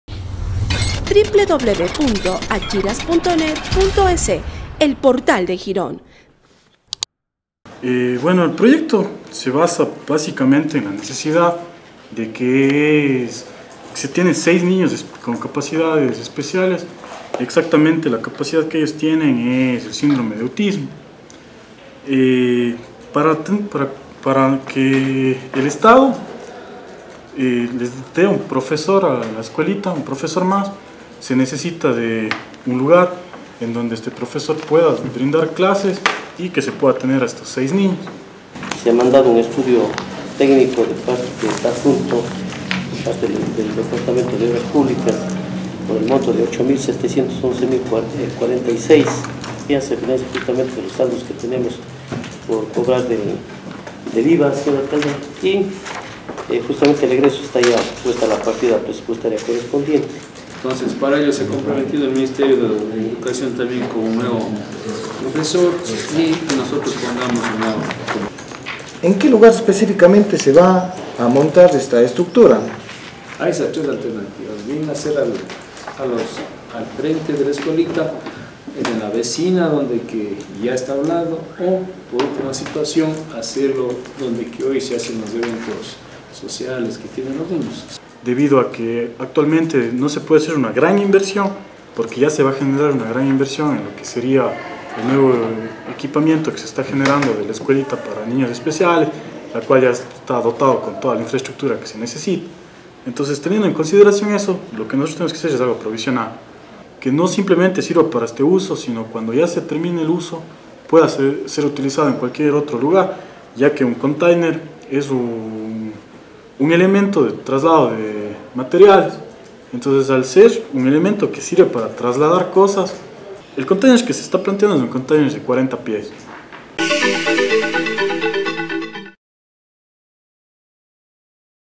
Aula para personas con autismo planifica construir el GAD Girón
En primera instancia y por unanimidad el concejo de Girón aprobó una nueva reforma al presupuesto 2017 por suplemento de crédito para la construcción de una nueva aula para acoger a seis niños con autismo, si se lo cumple, el Ministerio de Educación proveerá otro profesor. El proyecto de la aula se centra en adquirir un container y adaptarlo a la necesidad en forma provisional.